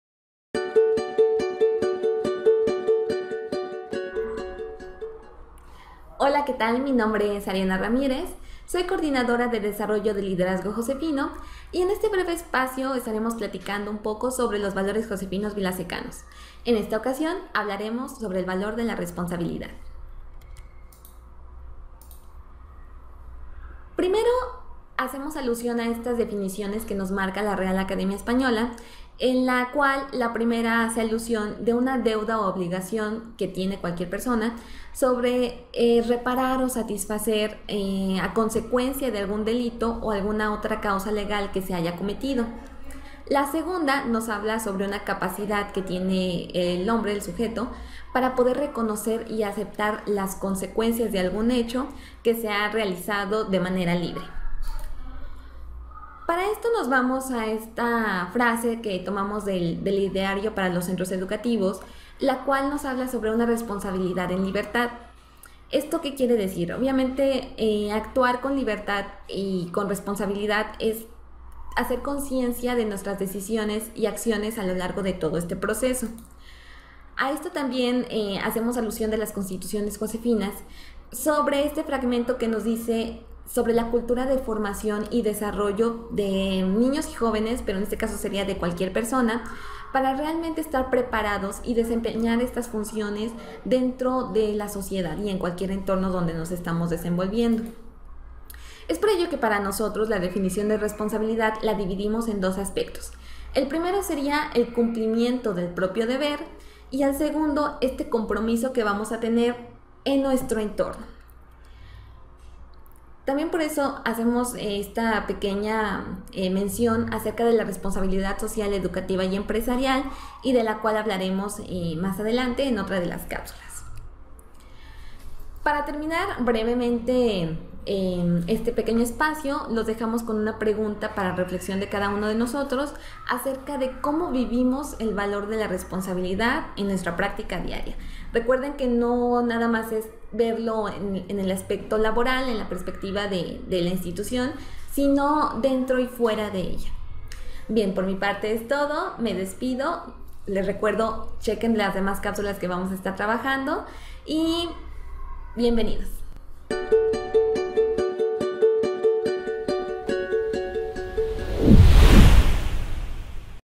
Valores Josefinos: Responsabilidad
En este video describimos el valor de la responsabilidad, que en nuestra institución hace énfasis en dos aspectos importantes:

1. El cumplimiento del propio deber y
2. El compromiso con el bien común de nuestro entorno.